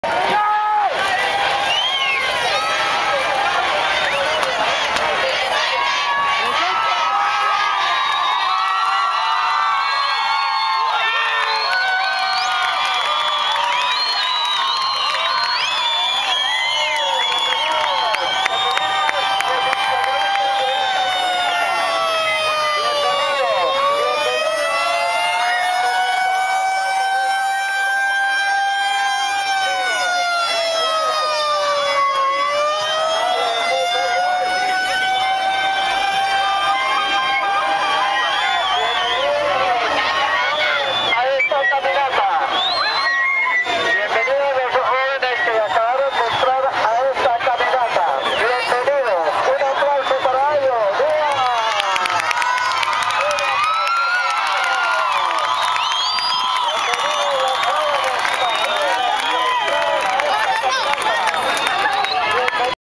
Marcha por el agua - Marcha por la vida - 01
Bucaramanga y su área metropolitana marchando en defensa del agua y de la vida. En contra de la aprobación de un proyecto minero que afectaría y contaminaria el ecosistema del páramo de Santurban. Amenazando además el abastecimiento de agua potable de los santandereanos